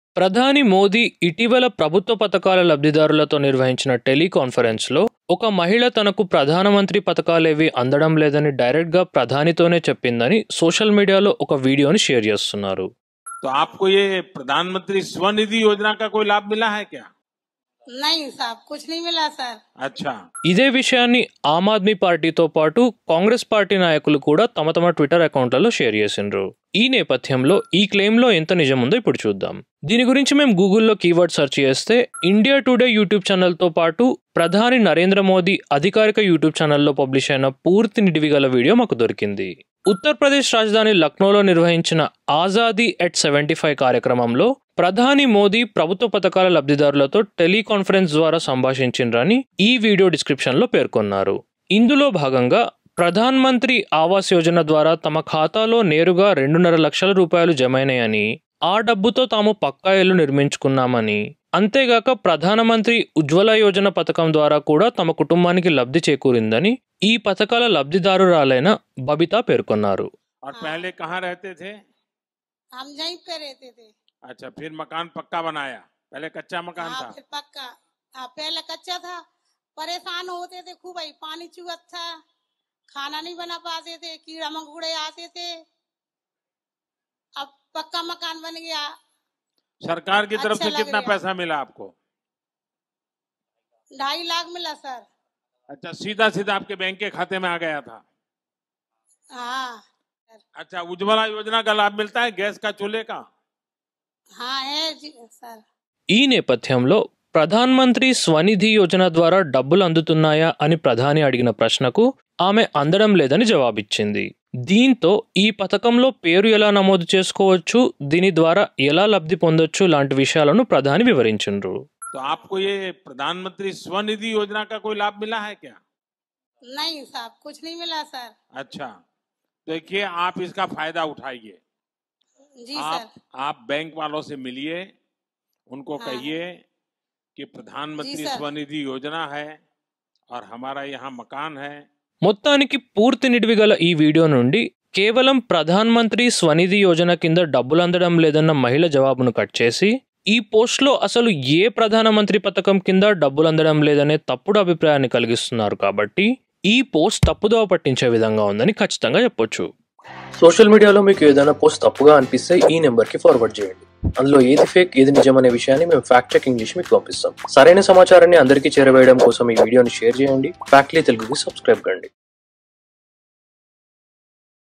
FACT CHECK: ప్రధాన మంత్రి పథకాల వల్ల ఏ మాత్రం లాభం లేదని డైరెక్ట్ గా ప్రధానితో పేర్కొన్న మహిళ?
ప్రధాన మంత్రి పథకాల వల్ల ఏ మాత్రం లాభం లేదని - ఒక మహిళ డైరెక్ట్ గా ప్రధానితో పేర్కొనిందన్న వార్తలో ఎంత నిజముందో ఈ వీడియో ద్వారా తెలుసుకోండి.